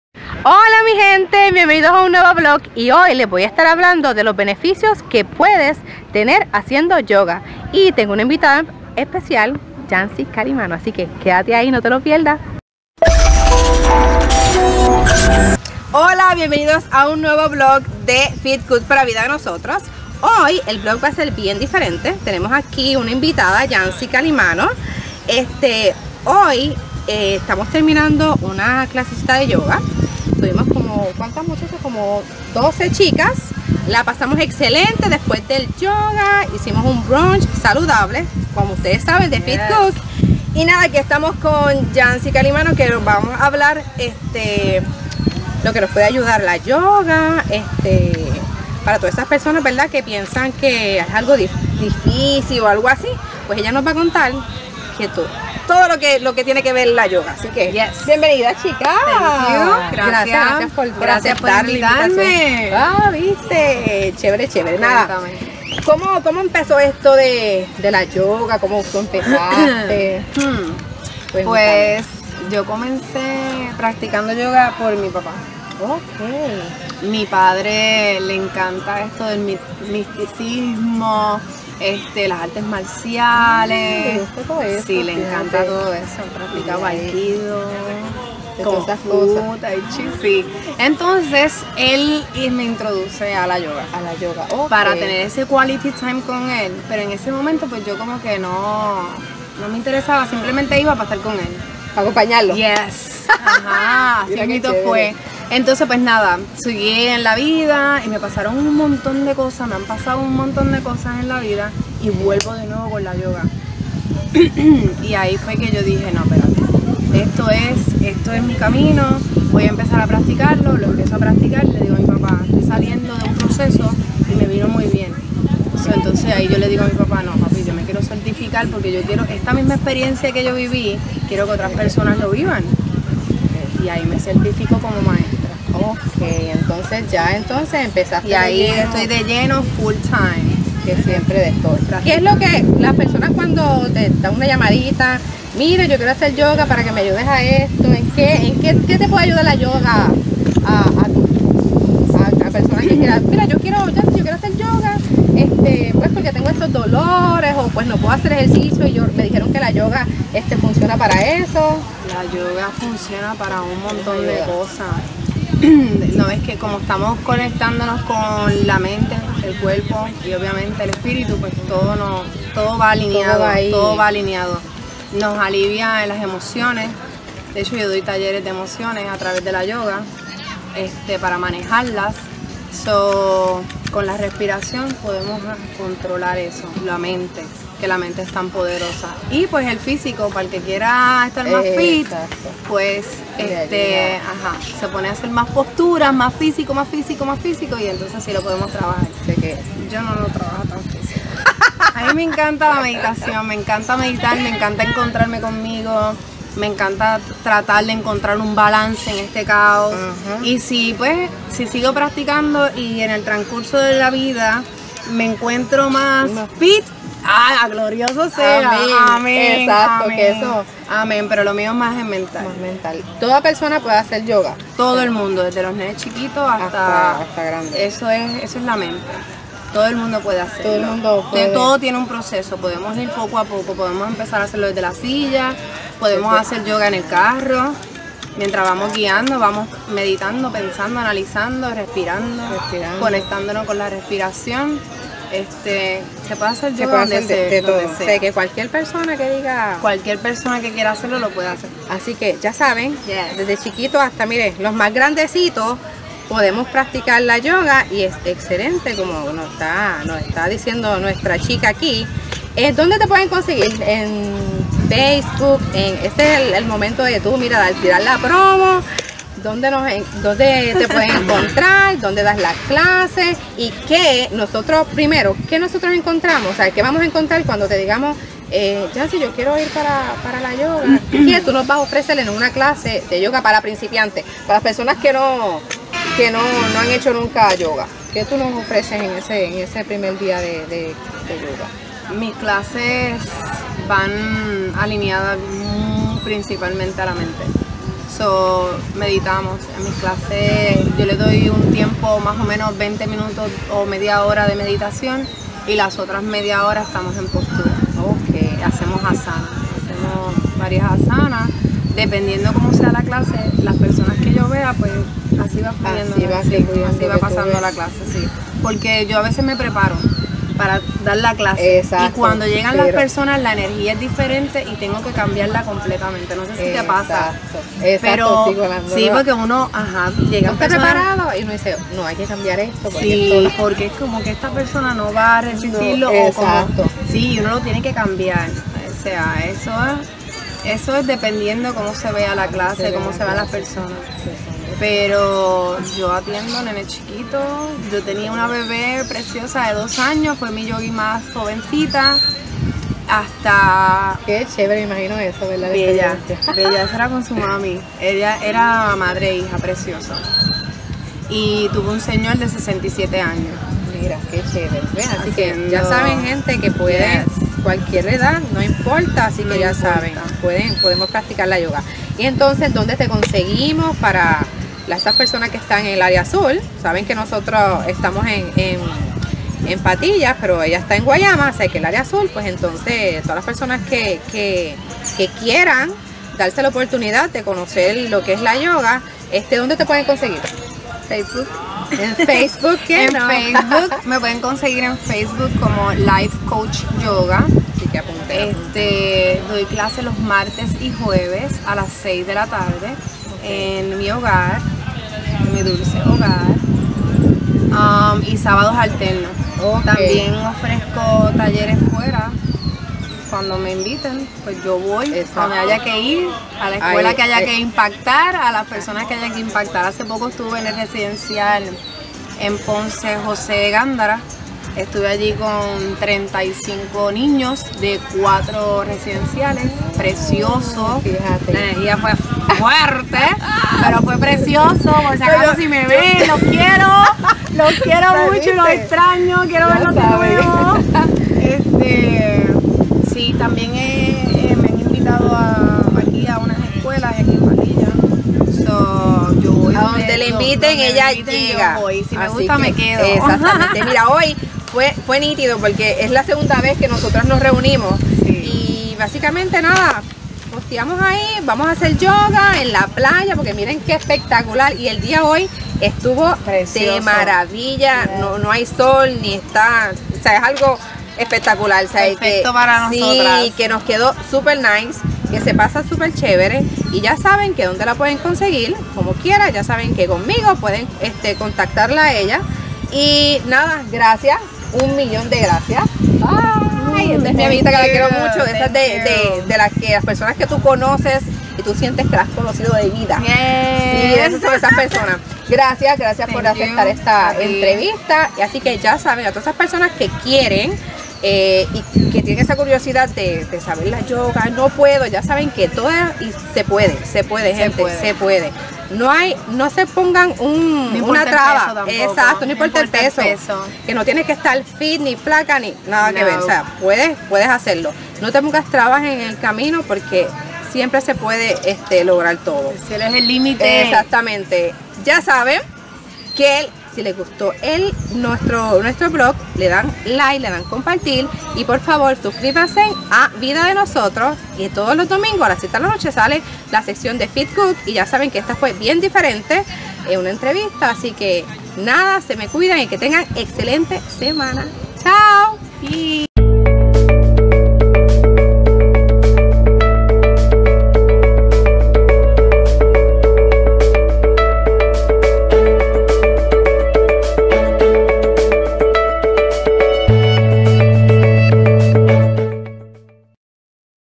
Para que son buenas las Clases de Yoga
El Yoga es Para Todos? - Entrevista a a Life Coach e Instructora de Yoga Yanzi Calimano. Una de esas entrevistas que si no conoces el Yoga, vas a entender el concepto y si piensas que no es para ti, te va animar a participar.